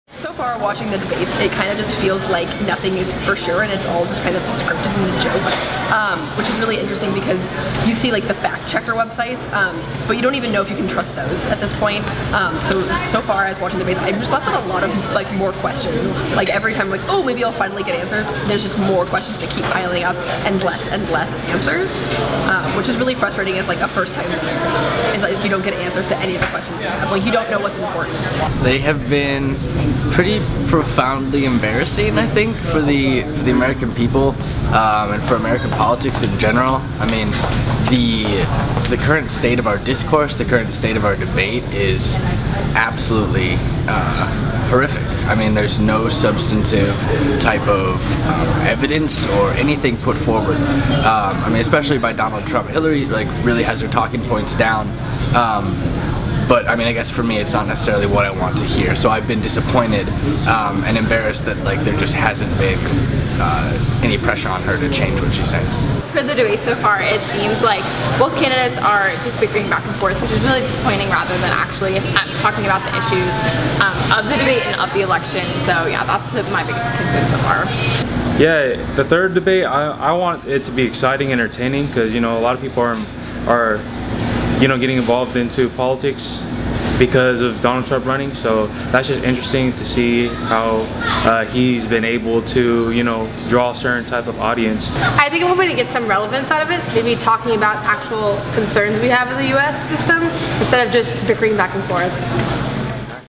Students Talk: Presidential Debates
Mast TV spoke with various students on the 2016 Presidential debates, and this is what they had to say.